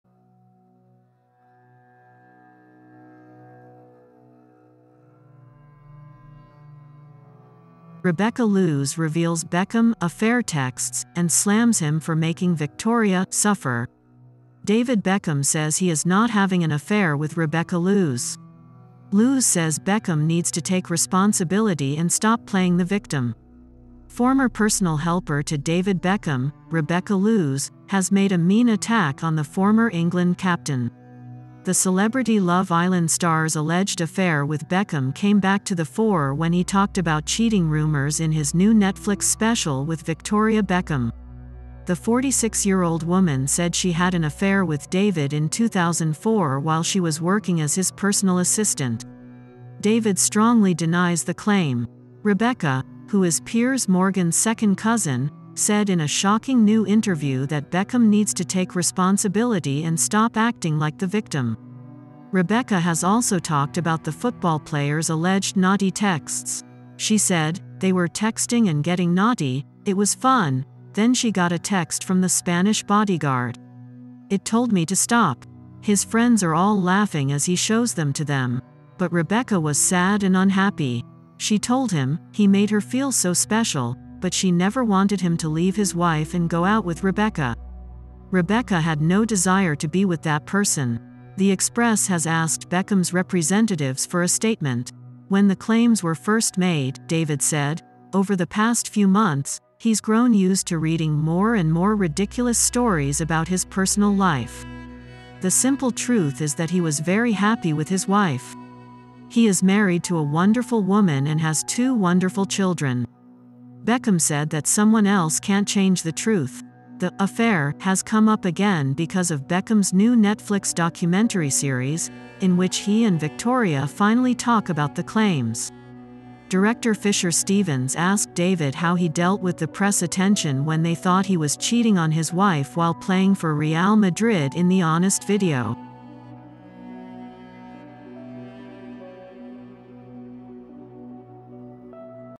Rebecca Loos reveals Beckham 'affair texts' and slams him for making Victoria 'suffer'
David Beckham says he is not having an affair with Rebecca Loos. Loos says Beckham needs to "take responsibility" and stop "playing the victim." Former personal helper to David Beckham, Rebecca Loos, has made a mean attack on the former England captain.

The Celebrity Love Island star's alleged "affair" with Beckham came back to the fore when he talked about cheating rumors in his new Netflix special with Victoria Beckham.